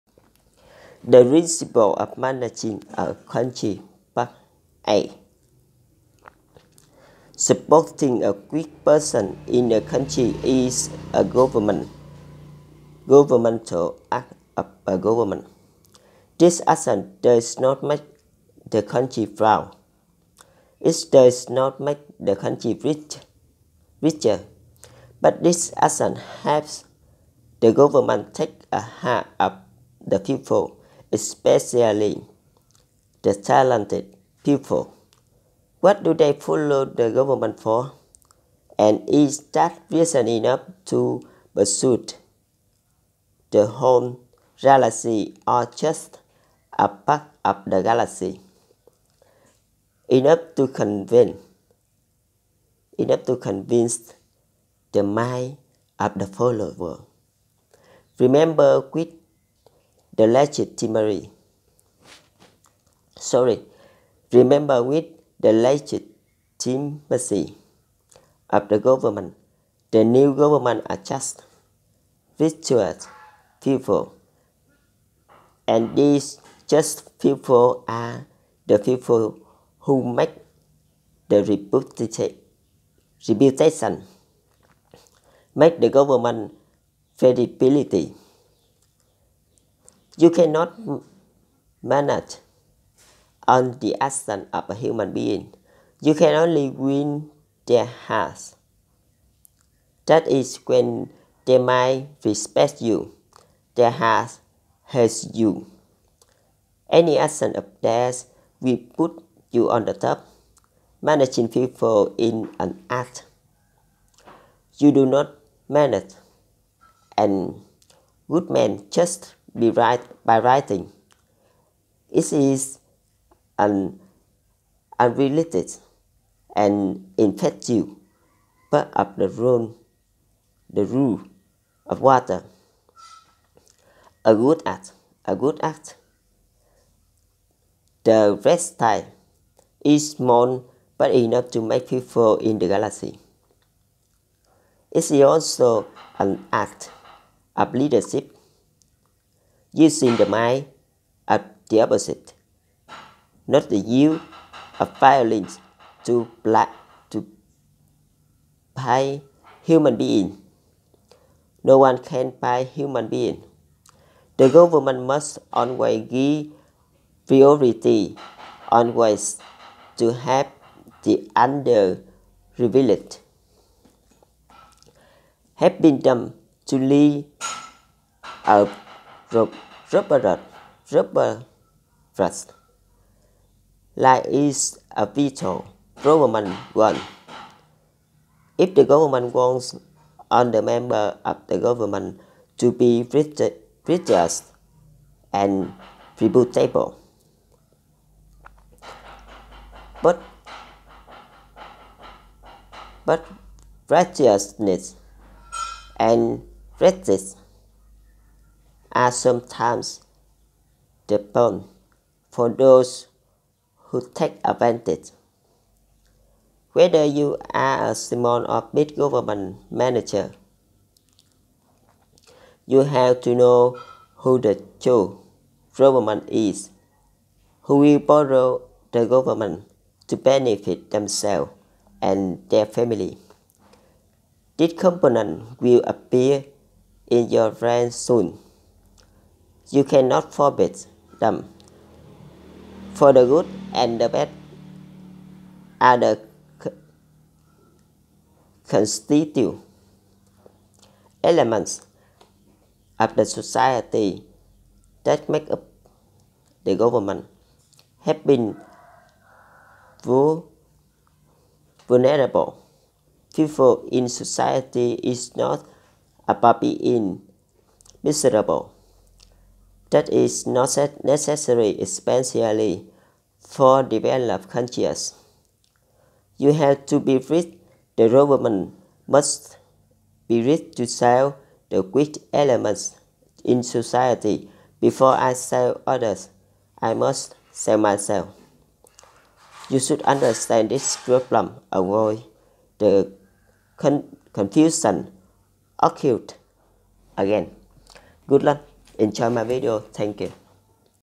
Social religion; the principla of managing a country part 8 (english)
Apoyar a los débiles en un país es un acto gubernamental. Esta acción no hace que el país crezca, no hace que el gobierno sea más rico. Pero esta acción ayuda al gobierno a tomar el corazón de la gente, especialmente de las personas con talento. ¿Para qué siguen al gobierno? ¿Y es esa razón suficiente para persuadir a toda la galaxia o solo una parte de la galaxia, suficiente para convencer a la mente de los seguidores?
Recuerde, con la legitimidad del gobierno, el nuevo gobierno atrae a personas justas. Y estas personas justas son las personas que hacen la reputación, que hacen que el gobierno sea creíble. No puedes gestionar todas las acciones de un ser humano. Solo puedes ganar sus corazones. Es decir, cuando su corazón te respeta, su corazón te tiene, cualquier acción de ellos te pondrá en la cima.
Manejar personas es un arte, no manejas a un buen hombre solo escribiendo. Es una parte poco realista e ineficaz de la regla del agua. Un acto justo, el prestigio es pequeño pero suficiente para hacer que la gente en la galaxia. También es un arte de liderazgo, usando la mente del contrario, no el uso de la violencia para atar a los seres humanos, nadie puede atar a los seres humanos.
El gobierno siempre debe dar prioridad, siempre para ayudar a aquellos que son débiles en la galaxia. Ayudarles a vivir una vida próspera es un objetivo vital del gobierno, si el gobierno quiere que todos los miembros del gobierno sean justos y de buena reputación.
Pero la justicia y el prestigio son a veces el cebo para aquellos que se aprovechan. Ya sea que sea un gerente de gobierno pequeño o grande, debe saber quién es el verdadero gobierno, quién prestará el gobierno para beneficiarse a sí mismos y a sus familias. Este componente aparecerá en sus filas pronto. No se les puede prohibir, porque lo bueno y lo malo son los elementos constitutivos de la sociedad que conforman el gobierno.
Ayudar a las personas vulnerables en la sociedad no se trata de ser miserable. Eso no es necesario, especialmente para los países desarrollados. Tienes que ser rico, el gobierno debe ser rico para salvar a los elementos débiles de la sociedad. Antes de salvar a otros, debo salvarme a mí mismo. Debes entender este problema, evita que la confusión vuelva a ocurrir.
Te deseo exito
PHAN MINH HAI DOCTRINA SOCIAL.